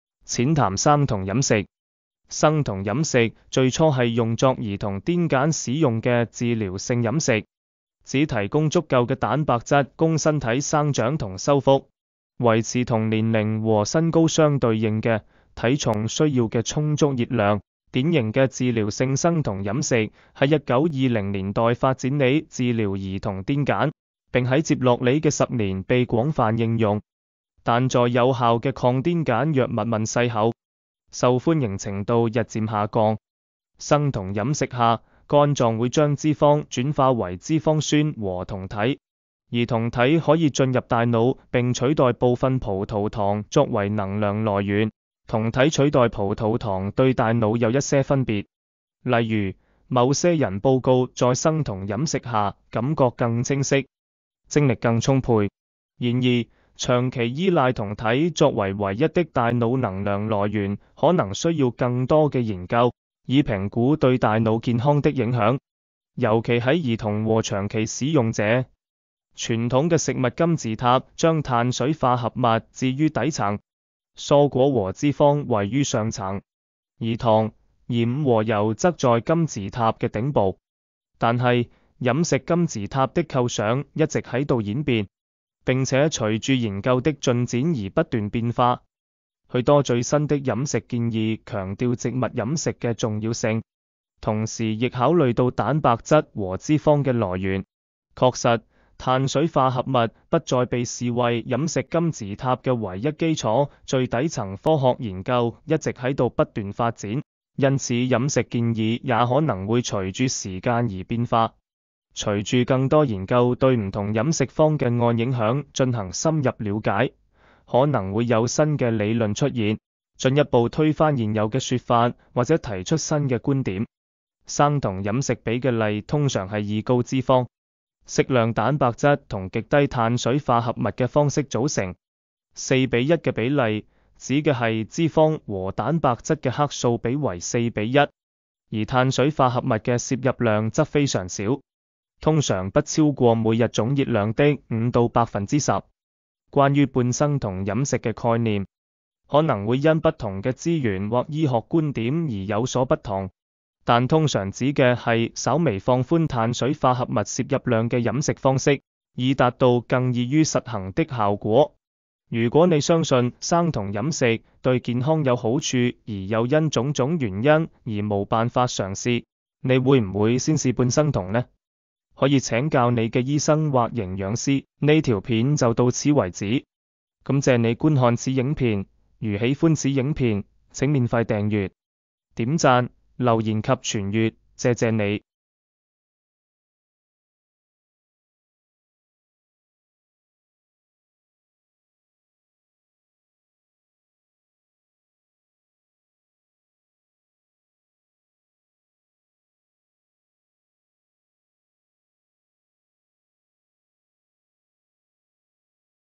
淺談生酮飲食 它最初係用作兒童癲簡使用嘅治療性飲食 颠覆了我們自幼對食物金字塔的認知
淺談生酮飲食

生酮飲食最初係用作兒童癲簡使用嘅治療性飲食，只提供足夠嘅蛋白質供身體生長同修復，維持（同年齡和身高相對應嘅）體重需要嘅充足熱量。典型嘅治療性生酮飲食. 喺1920年代發展梨治療兒童癲簡，並喺接落梨嘅十年被廣泛應用，但在有效嘅抗癲簡藥物問世後，受歡迎程度日漸下降。

生酮飲食下，肝臟會將脂肪轉化為脂肪酸和酮體，而酮體可以進入大腦並取代部分葡萄糖作為能量來源。酮體取代葡萄糖對大腦有一些分別，例如，某些人報告在生酮飲食下感覺更清晰、精力更充沛。然而，長期依賴酮體作為唯一的大腦能量來源可能需要更多嘅研究，以評估對大腦健康的影響，尤其喺兒童和長期使用者。

傳統嘅食物金字塔將碳水化合物置於底層，蔬果和脂肪位於上層，而糖、鹽和油則在金字塔嘅頂部。

但係，飲食金字塔的構想一直喺度演變，並且隨著研究的進展而不斷變化。許多最新的飲食建議強掉植物飲食嘅重要性，同時亦考慮到蛋白質和脂肪嘅來源。確實，碳水化合物不再被視為飲食金字塔嘅唯一基礎 (最底層)。

科學研究一直喺度不斷發展，因此飲食建議也可能會隨著時間而變化。隨著更多研究對唔同飲食方嘅案影響進行深入了解，可能會有新嘅理論出現，進一步推翻現有嘅說法或者提出新嘅觀點。

生酮飲食比嘅例通常係以高脂肪、適量蛋白質同極低碳水化合物嘅方式組成。4比1嘅比例,指嘅係脂肪和蛋白質嘅克數比為4比1，而碳水化合物嘅攝入量則非常少，通常不超過每日總熱量的5到 10%。關於半生酮飲食嘅概念，可能會因不同嘅資源或醫學觀點而有所不同，但通常指嘅係稍微放寬碳水化合物攝入量嘅飲食方式，以達到更易於實行的效果。

如果你相信生酮飲食對健康有好處而又因總總原因而無辦法嘗試，你會唔會先試半生酮呢？可以請教你嘅醫生或營養師。

呢條片就到此為止。